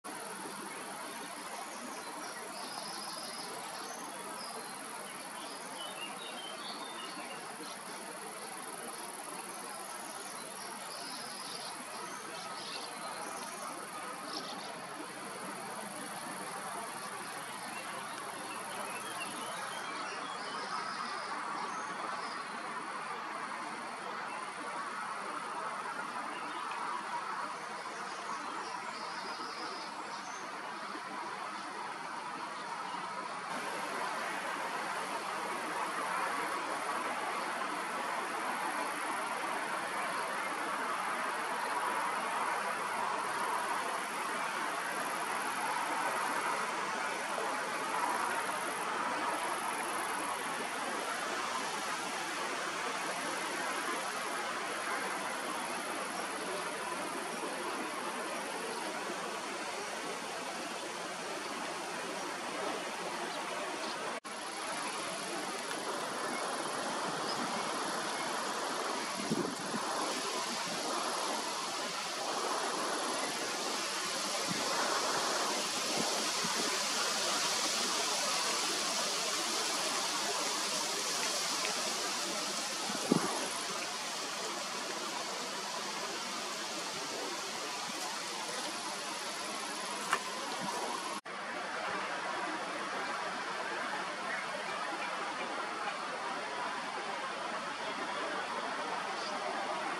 Black stork - Schwarzstorch - Ciconia nigra
Black stork - Schwarzstorch - Ciconia nigra - feeding and resting at nature preserve Altrhein-Wyhlen (DE), July 2019